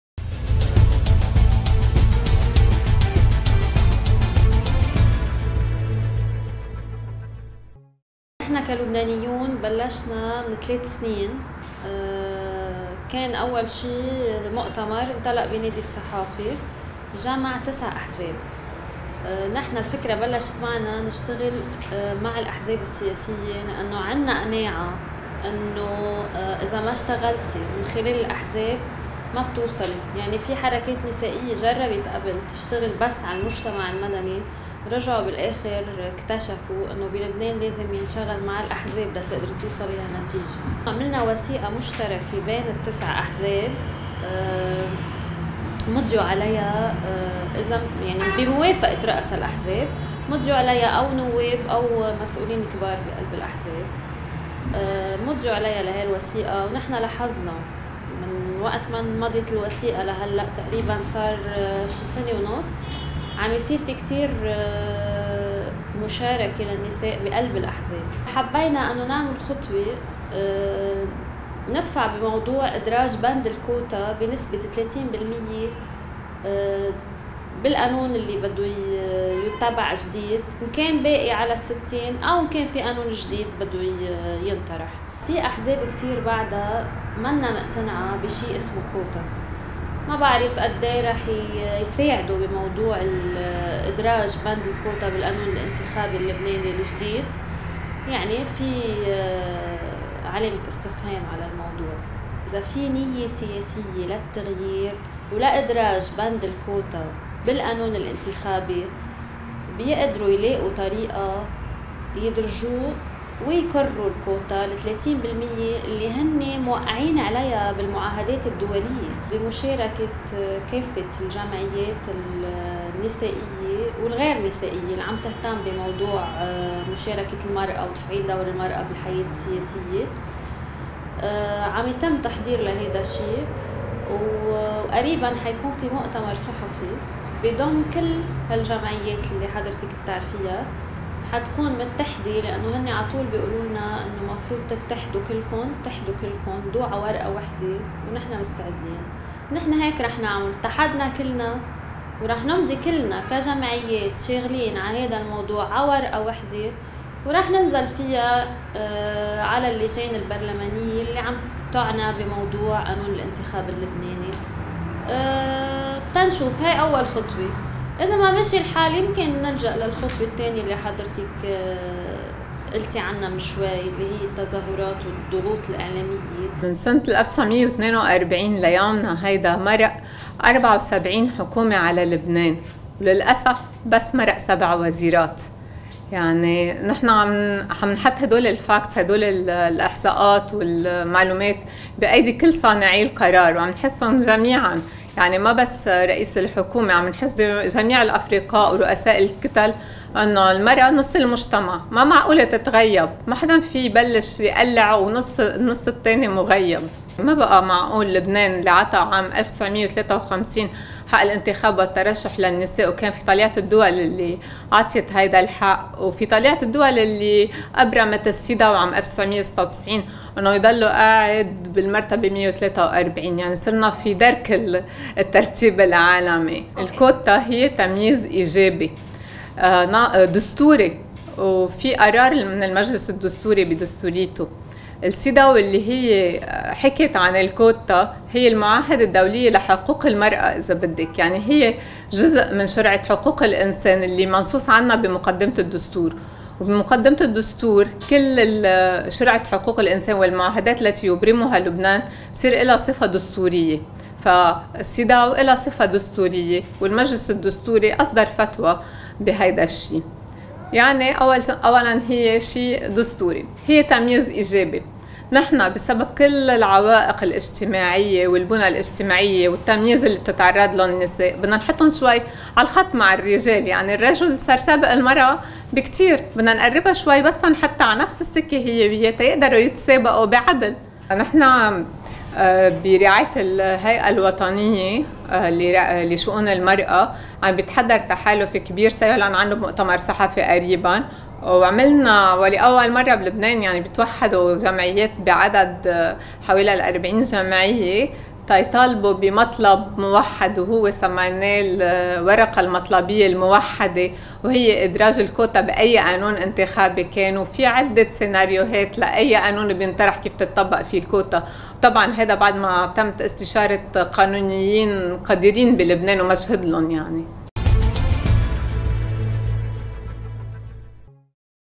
مصر العربية | الكوتا السياسية.. خطوة مطلوبة باتجاه تمكين المرأة اللبنانية‎‎
رغم توقيع لبنان على المواثيق الدولية التي تكفل حق مشاركة المرأة في الحياة السياسية، إلا أن هذا البلد العربي يقبع في المرتبة قبل الأخيرة 143/ 144 من ناحية التمثيل السياسي للنساء، بحسب تقرير "الفجوة بين الجنسين"، الذي أصدره منتدى الاقتصاد العالمي في أكتوبر/ تشرين أول الماضي.